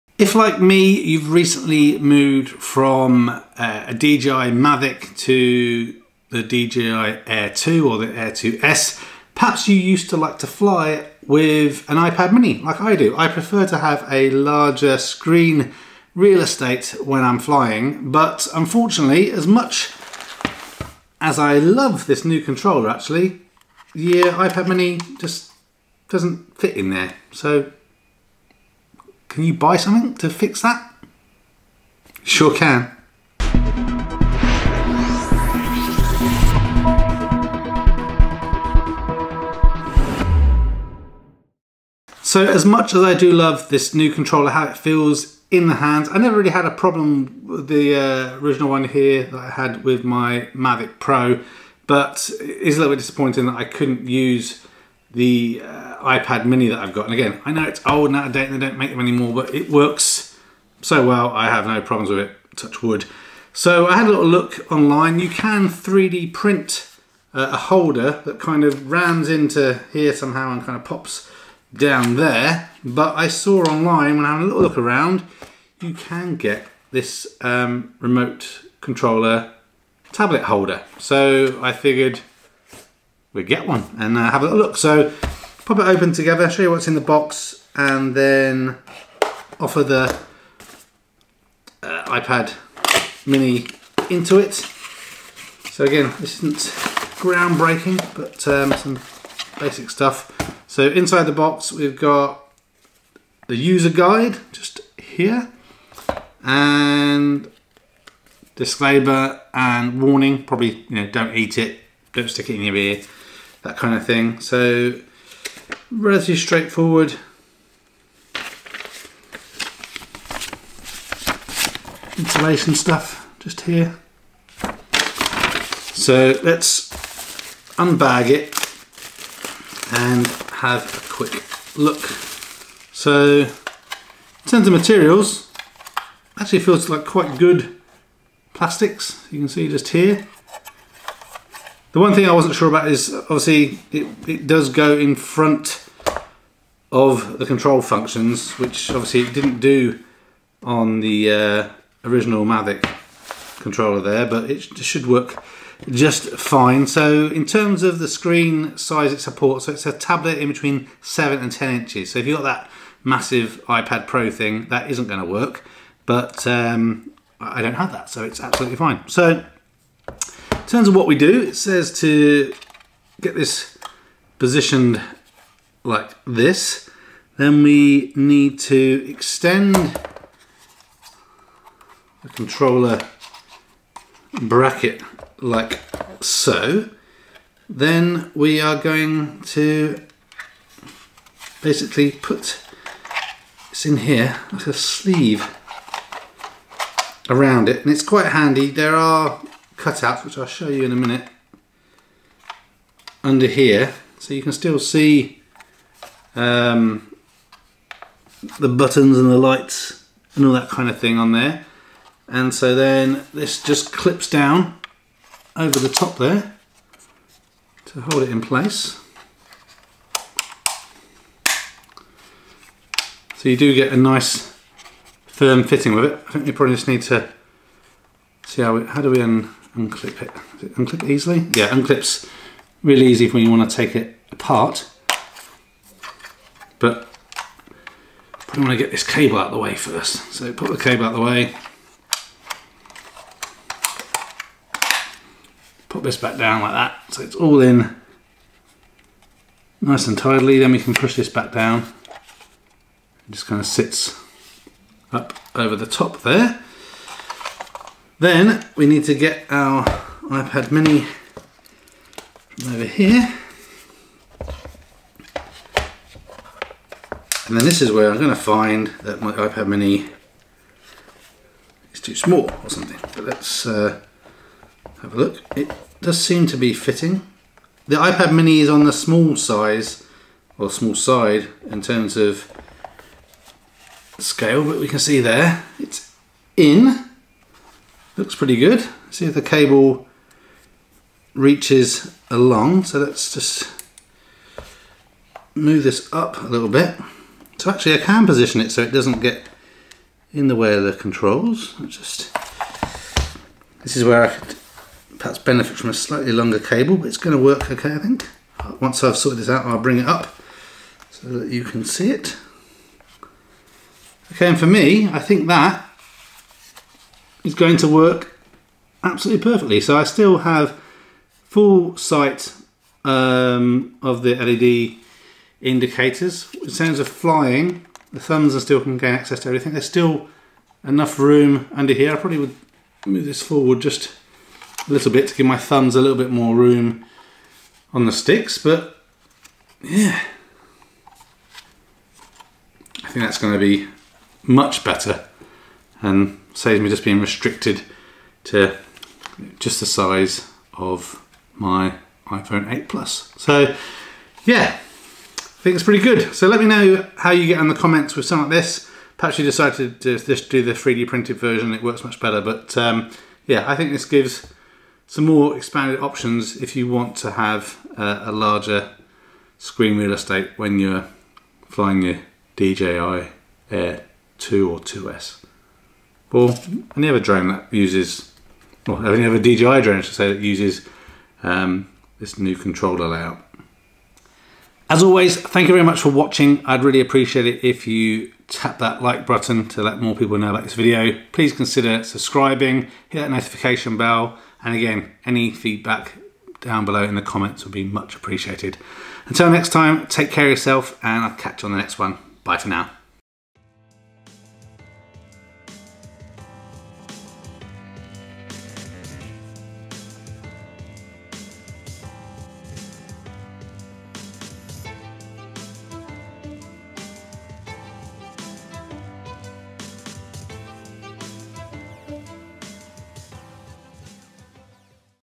Checking out the DJI Tablet Holder
I wanted to be able to use my old Apple iPad Mini with my new DJI Air 2S, so I picked up the official DJI Tablet Holder to see if its any good.

Timecodes